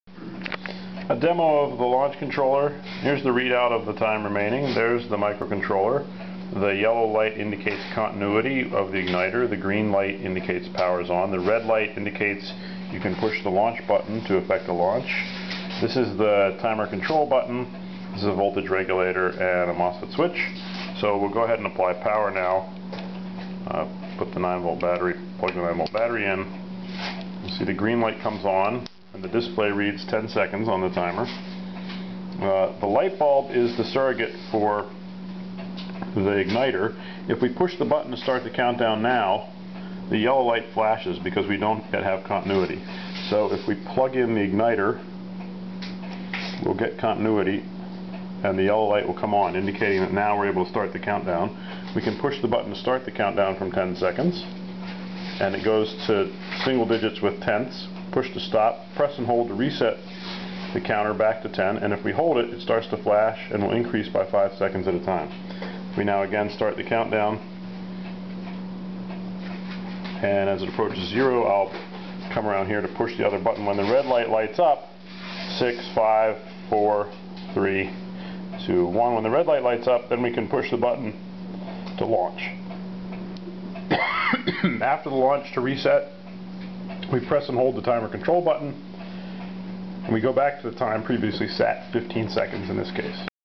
Model Rocket Launch Controller with MaEvArM
Project to replace Estes rocket launch controller.  Demonstration project making extensive use of reusable code libraries to allow high-level control of digital I/O and user interrupts using only Timer0.